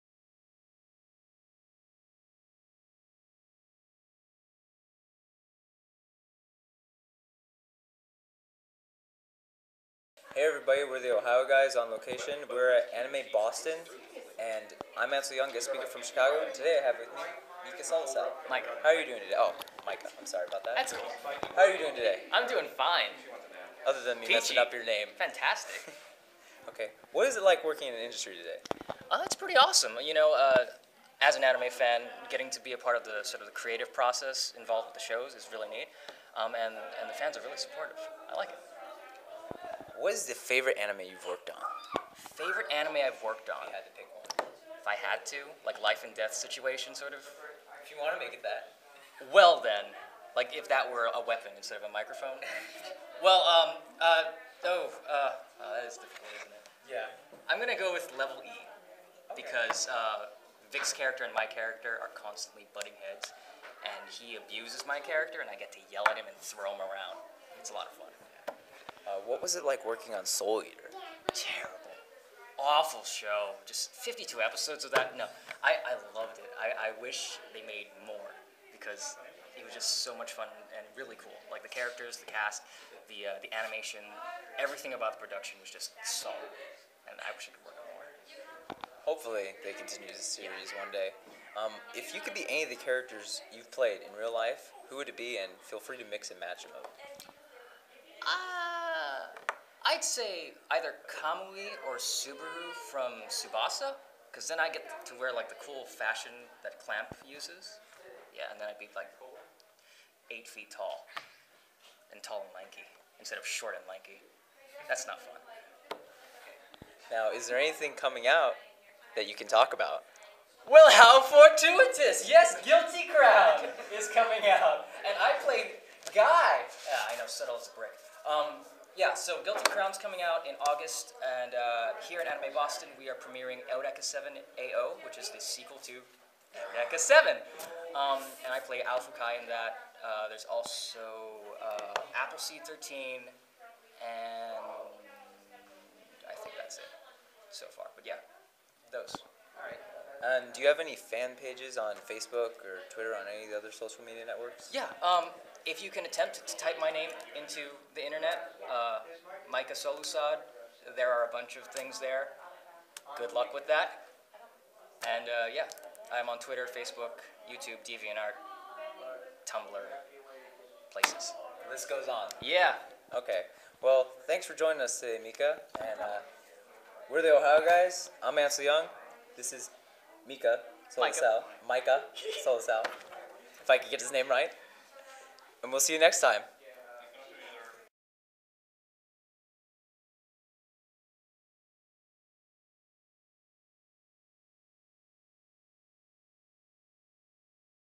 Voice of the Voices - Micah Solusod
It is finally time another edition of Voice of the Voices. Tonight's guest is none other than Mr. Micah Solusod

1. Comment on our videos or website - we do give shoutouts